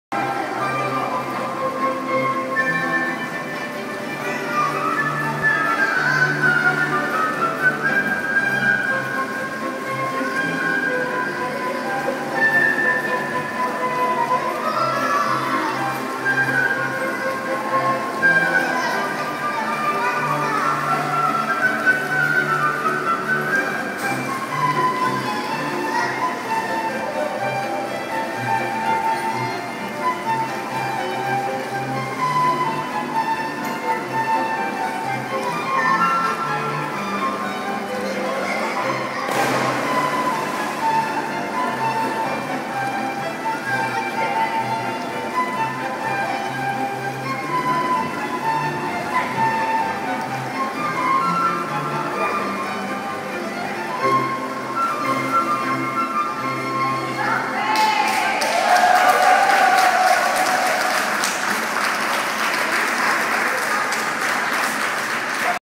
Concert à l'eau La Journée enchantée
15h : Bal Mus'Art offre un grand bain de musique dans le centre nautique de Coursaille (Aulnay). Après deux sets pour les nageurs et le public assis dans les gradins, c'est le moment d'animer une séance de gym aquatique !
Avec l'ensemble Bal Mus'Art : Rachel Averly (flûte traversière), Laurence Dupré (violon), Laurence Jordan (contrebasse), Noëllie Nioulou (violoncelle) et Olivier Wely (alto).
Enregistré lors de La Journée enchantée, samedi 19 octobre 2013. © Théâtre Jacques Prévert - S. Daniel